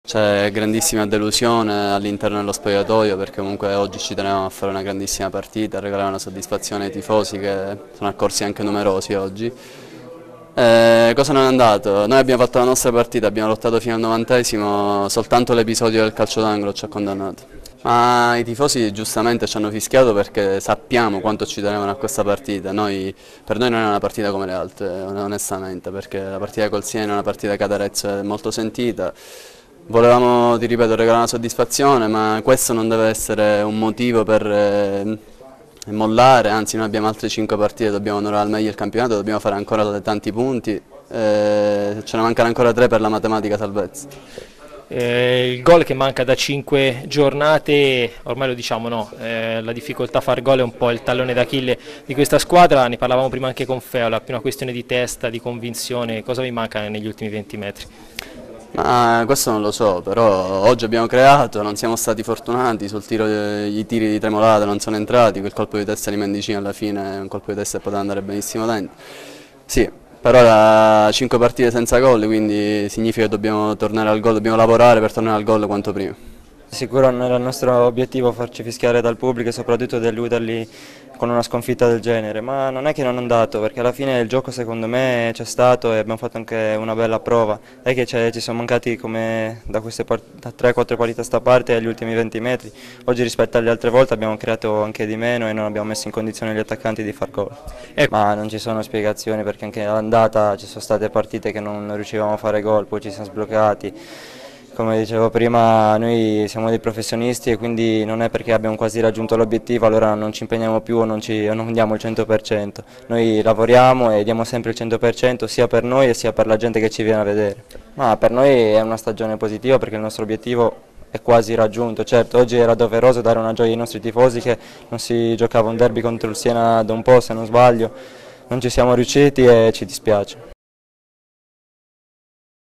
Calcio, Arezzo--Siena 0-1: interviste a Feola e Sabatino
Le interviste a Feola e Sabatino dopo il derby con il Siena